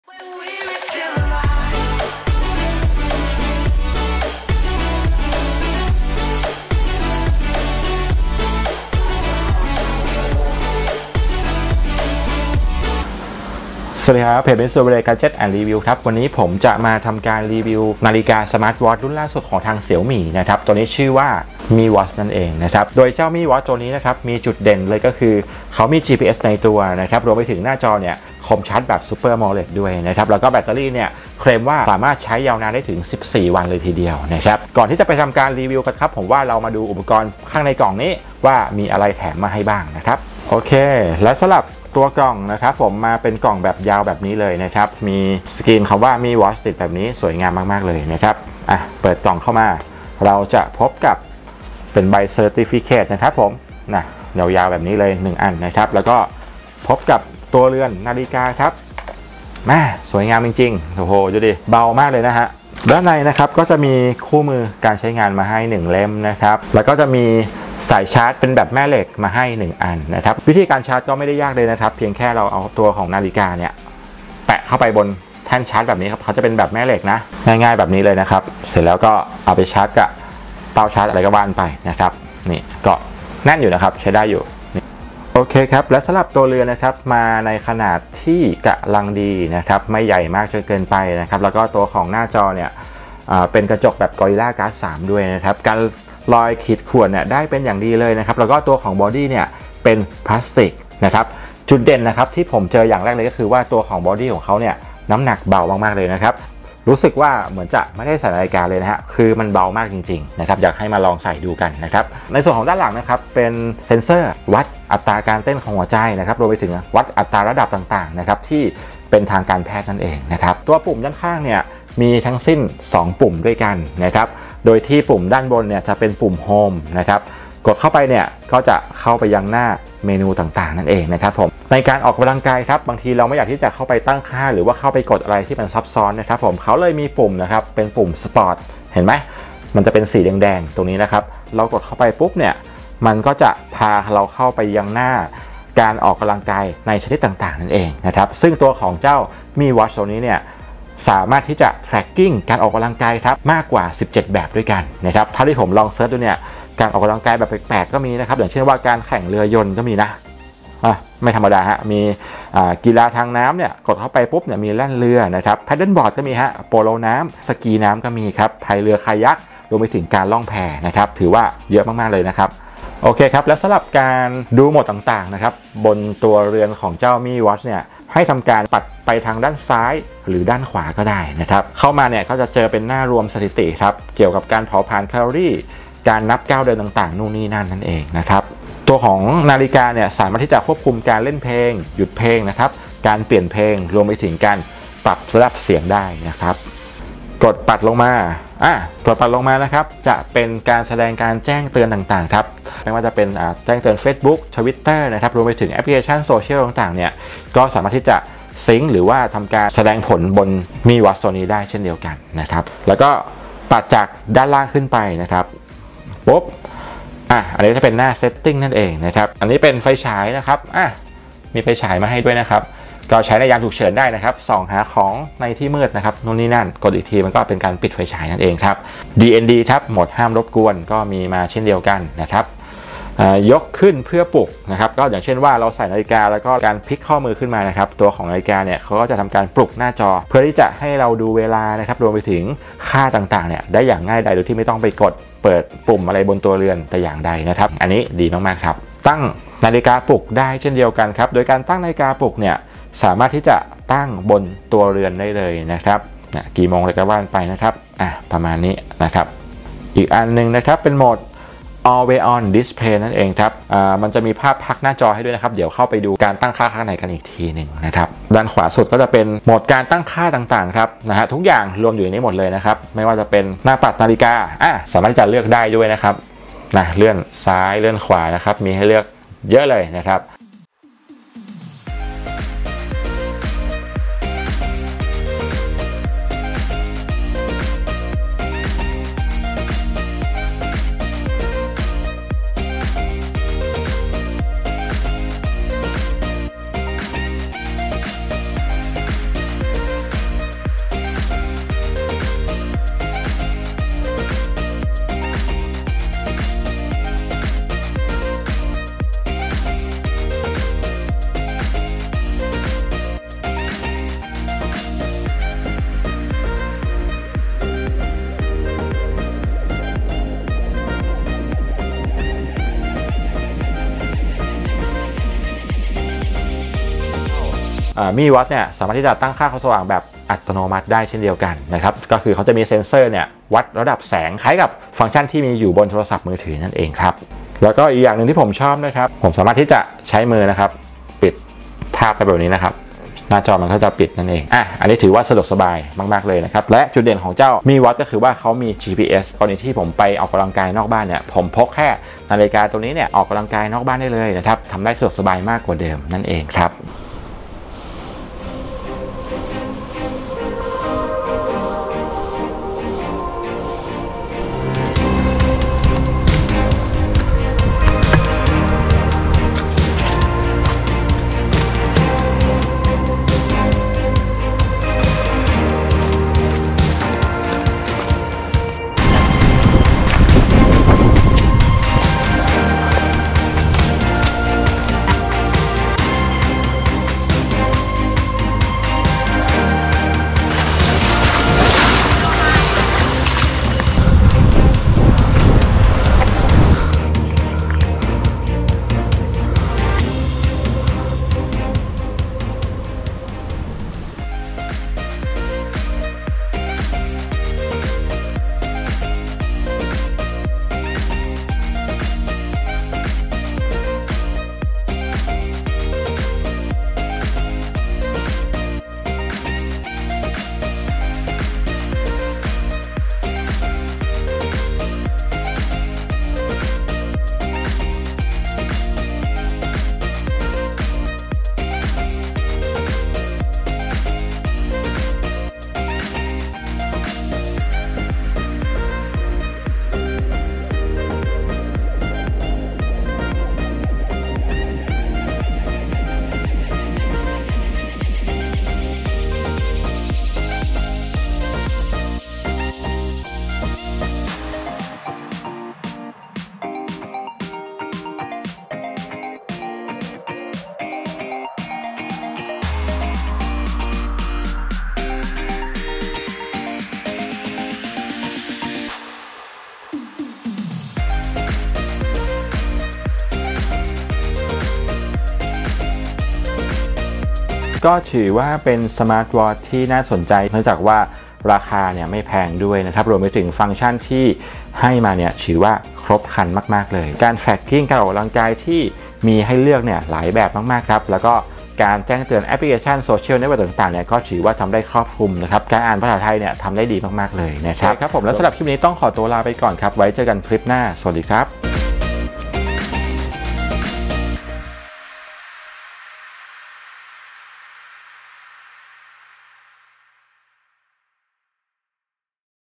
Review Mi watch :Nice Budget Smart Watch Full Function
Review Xiaomi Mi Watch  : Smart watch ราคาดี ฟังก์ชั่นเยอะ "พร้อมทดสอบจริงภาคสนามแบบจัดเต็ม !!
ข้อมูลตัวเครื่อง
สมาร์ทวอทช์ (นาฬิกาอัจฉริยะ Smartwatch)
หน้าจอ จอแสดงผล (450 นิต)
- หน้าจอ วงกลม
- กระจกหน้าจอ Corning Gorilla Glass 3
- ระบบสัมผัส Multi-Touch
- จอภาพ AMOLED
ขนาด 1.39 นิ้ว
- กว้าง 45.9 มม. (แนวทะแยง) x ยาว 53.3 มม. x หนา 11.8 มม.
- พื้นที่แสดงผล 6.2 ตร.มม.
- ความละเอียด 454 x 454 พิกเซล
มี GPS ในตัว สามารถใช้ Tracking การออกกำลังกายโดยไม่ต้องต่อกับโทรศัพท์มือถือ
- มีสีให้เลือก (Colors) Blackสายนาฬิกา
- ประเภทสาย สาย TPU
แบตเตอรี่
- ใช้งานต่อเนื่อง 16 วัน
ระบบ Tracking การออกกำลังกายครบครันมากกว่า 20 รายการ
VO2max , Heart Rate , และระบบติดตามสุขภาพแบบต่างๆ
เชื่อต่อ Application Xiao Wear
ในราคาเพียง 3,490.- ที่นี่เลย